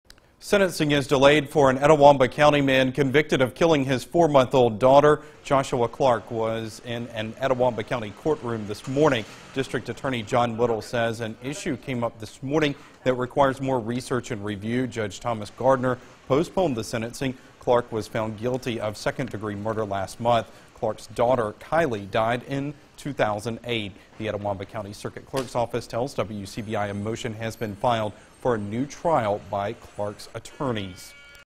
Sentencing Delay
Sentencing is delayed for an Itawamba County man convicted of killing his four month old daughter.